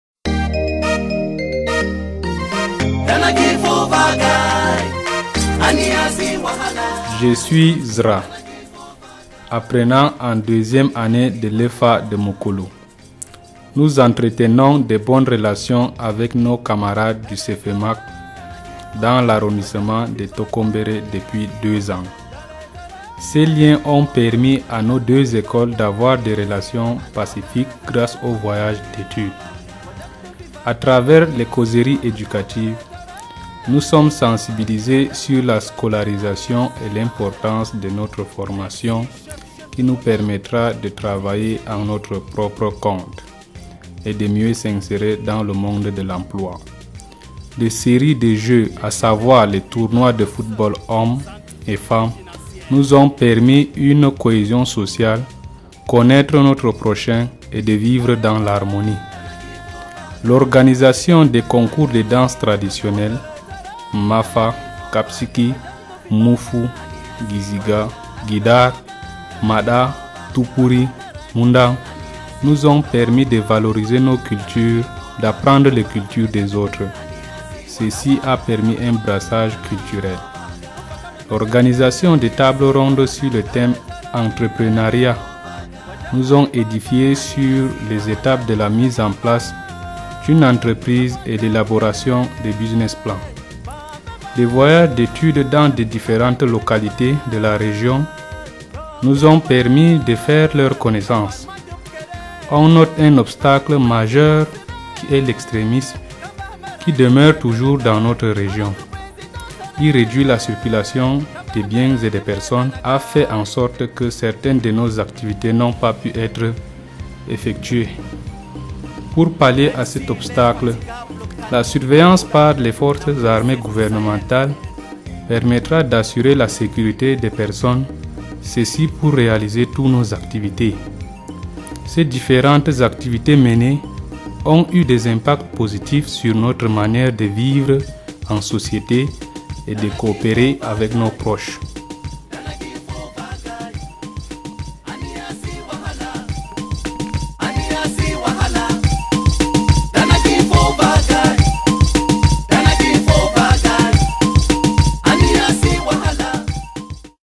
Les Relations Pacifique Entre CEFFA Mak Et EFA+
Ses différentes  activités menées dans les exchange ont eu des impacts positifs sur notre manière de vivre en société et de coopérer avec nos proches.
Des voyages d’études dans des différentes localités de la région nous ont permis de faire leur connaissance.